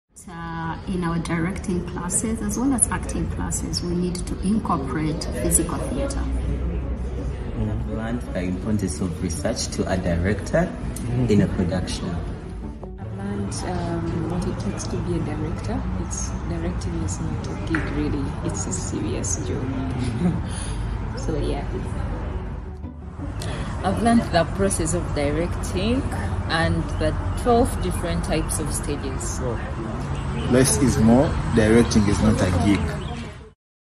Directing Workshop by Asiimwe Deborah Kawe Review
We surprised (the Stage) Directing Workshop students & their amazing tutor
Asiimwe Deborah Kawe to spill the tea on their learnings (and maybe some stage bloopers!)